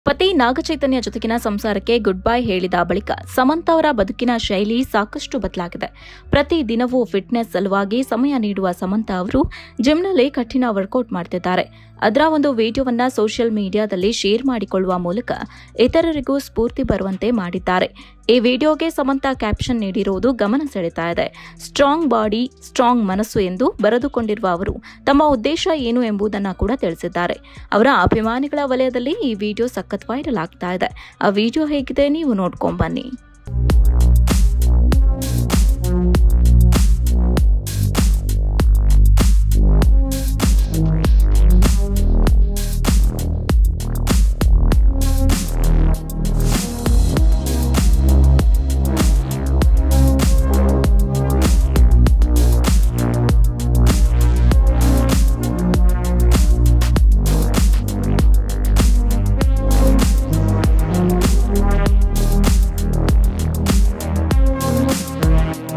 Samantha fitness | ಸ್ಟ್ರಾಂಗ್ ಬಾಡಿ, ಸ್ಟ್ರಾಂಗ್ ಮನಸ್ಸು ಎಂದು ಸಂದೇಶ ಸಾರಿದ ಸಮಂತಾ
ನಟಿ ಸಮಂತಾಗೆ ಫಿಟ್‌ನೆಸ್ ಅಂದ್ರೆ ಎಲ್ಲಿಲ್ಲದ ಪ್ರೀತಿ. ಇದೀಗ ಮತ್ತೊಂದು ಸಾಹಸಮಯ ವೀಡಿಯೊವನ್ನು ಸಮಂತಾ ಹಂಚಿಕೊಂಡಿದ್ದಾರೆ, ಈ ಮೂಲಕ ಅಭಿಮಾನಿಗಳಿಗೆ ಅಚ್ಚರಿ ಮೂಡಿಸೋದಲ್ಲದೇ, ಸಂದೇಶವನ್ನು ನೀಡಿದ್ದಾರೆ.